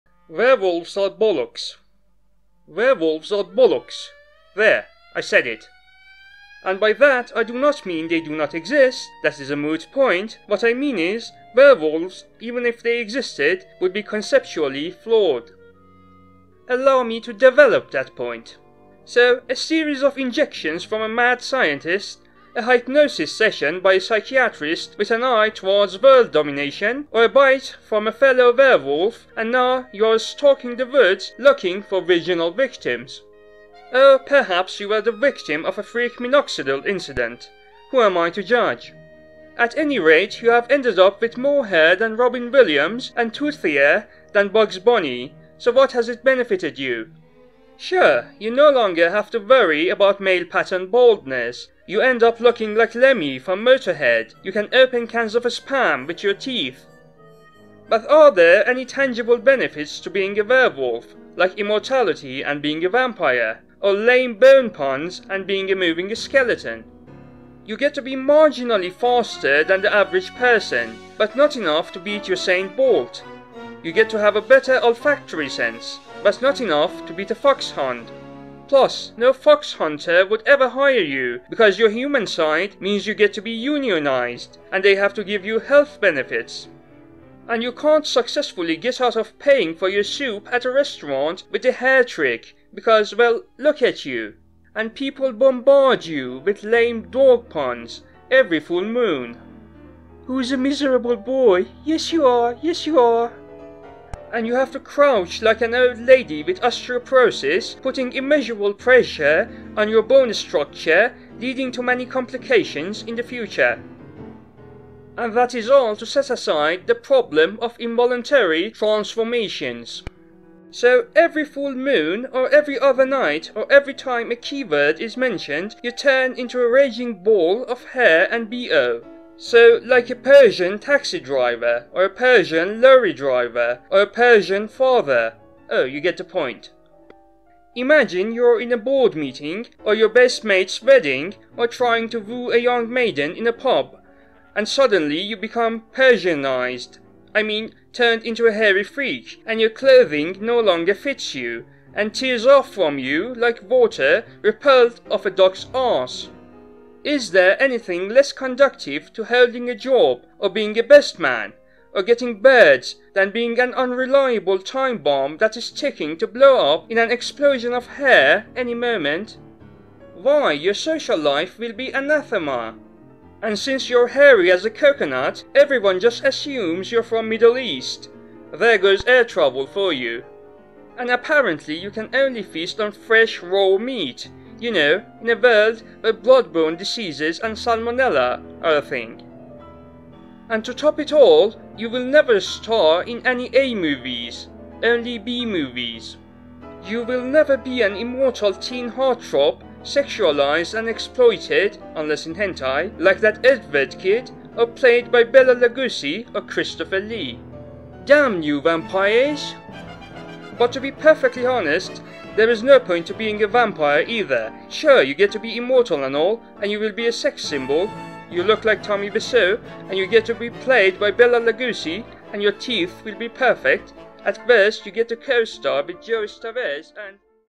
Why Being a Werewolf would Suck - A Wanton Thought
Sit back with a cup of tea, and idly listen to some Greshwin in the background as Uncle H tells you why werewolves are a load of old cobblers.